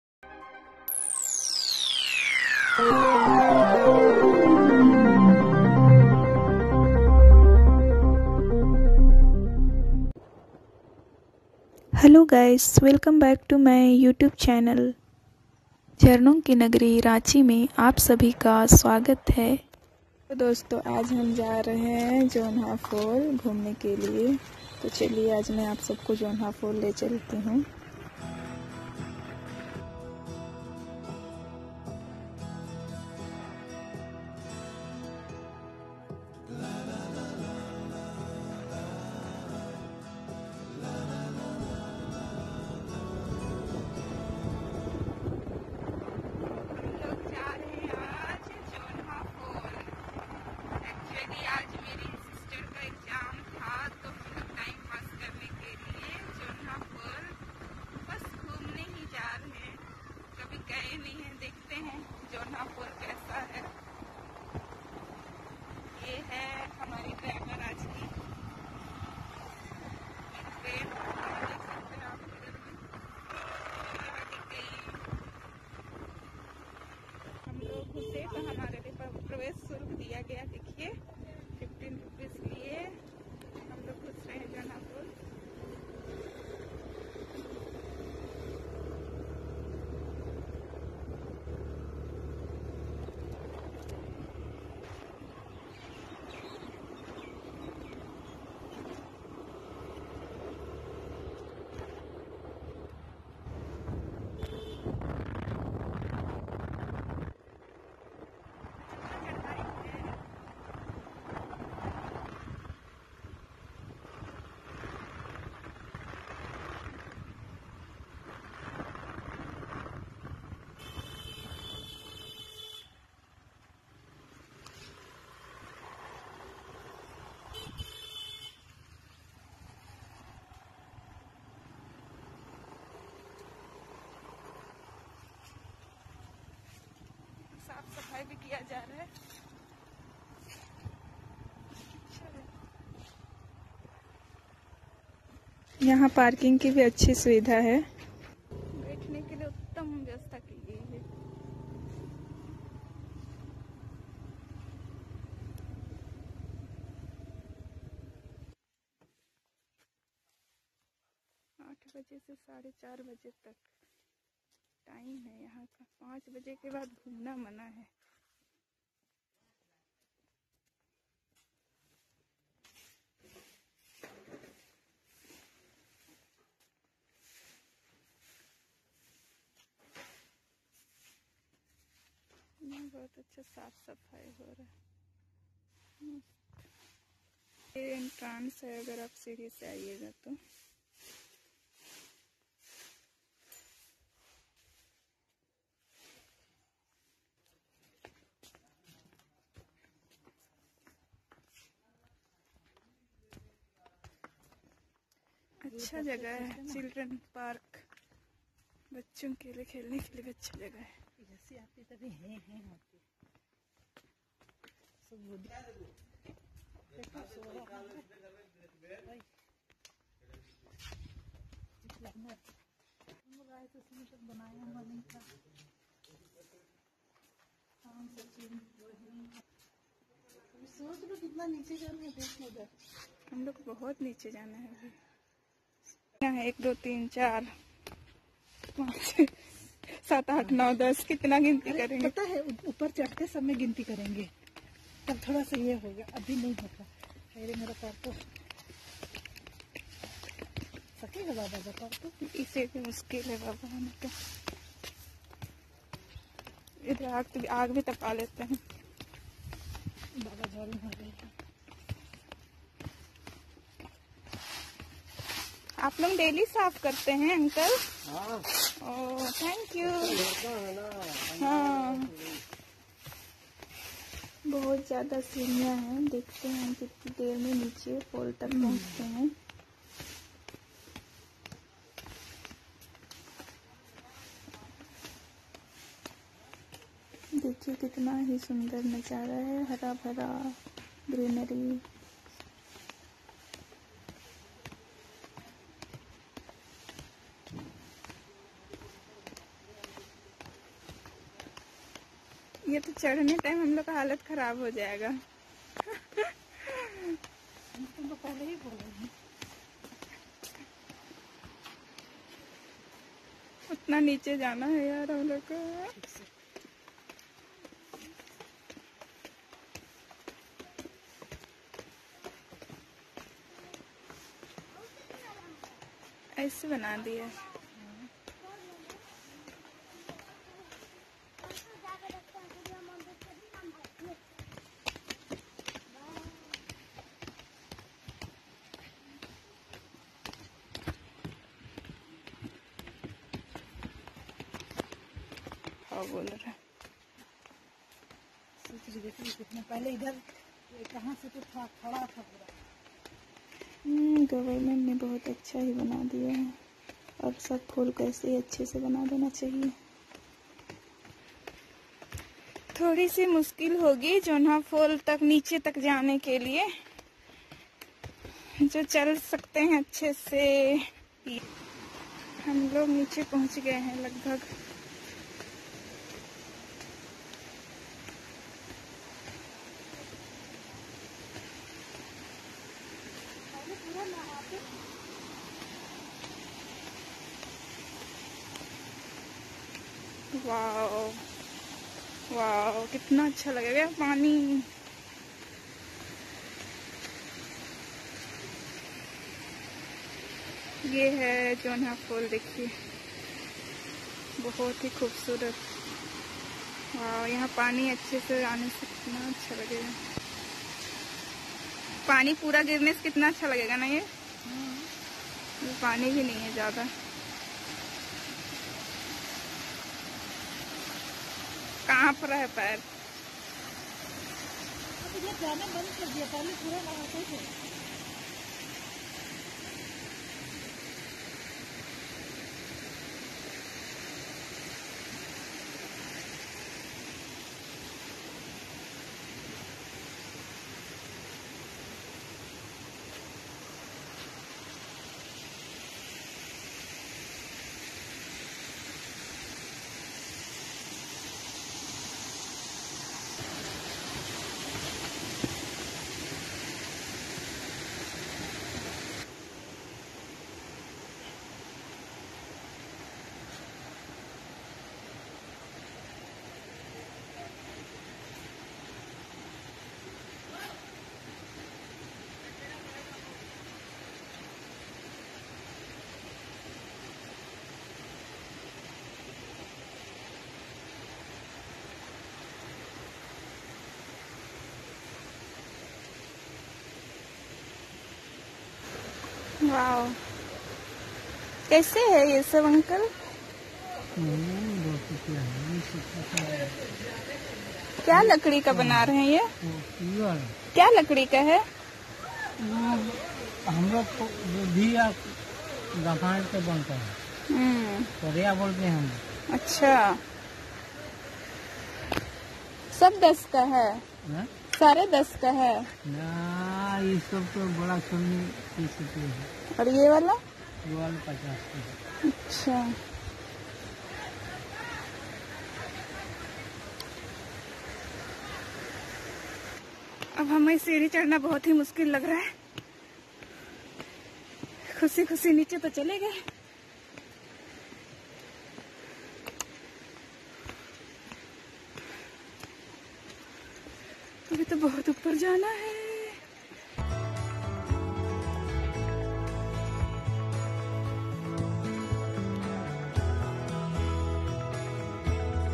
Jonha Fall/ Ranchi, Jharkhand/जोन्हा फाॅल/
Jonha Fall
    About 45 kms away from Ranchi situated on the Ranchi-Purulia Highway, is the Jonha Falls, named after the local village. It is also known as the Gautamdhara, as there is a temple dedicated to Lord Budha, in its vicinity.

A very nice falls.. Its so beautiful and natural place to enjoy. A must visit if you are in the town.

Beautiful place. Enjoyed with friends. Environment was cool and attractive. There was nice fall and running in beautiful way.